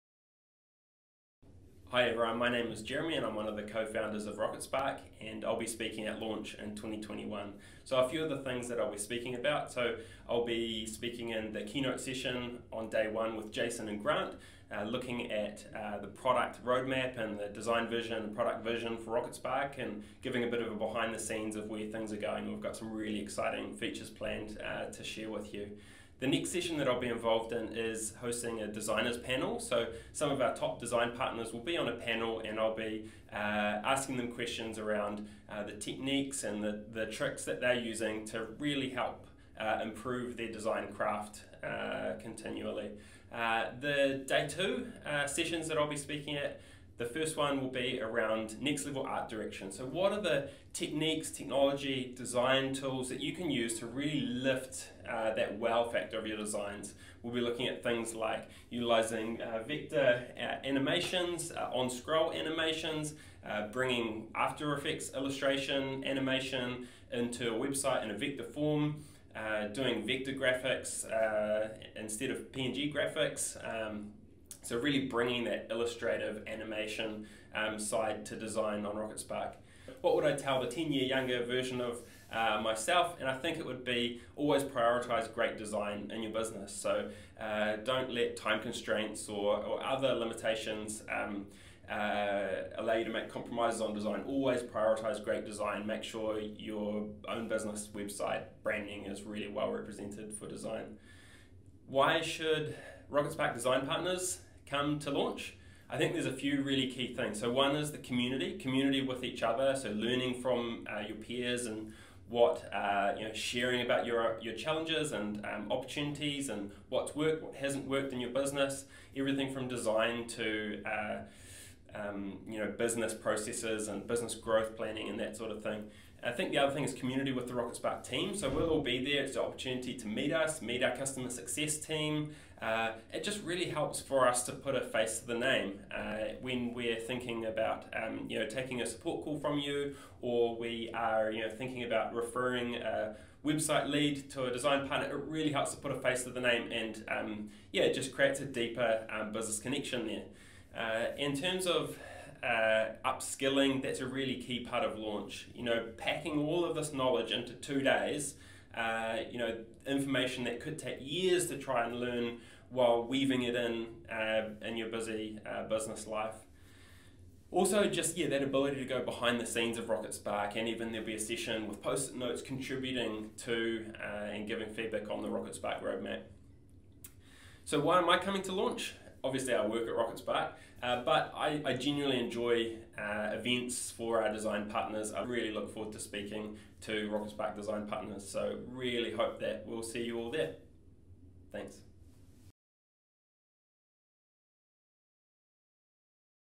Jeremy Johnson on Launch 2021
Before co-founding Rocketspark, Jeremy was a freelance graphic designer. At Launch, Jeremy will talk about product development, award-winning design, design direction, a deep dive into better visual merchandising for e-commerce, and product insights.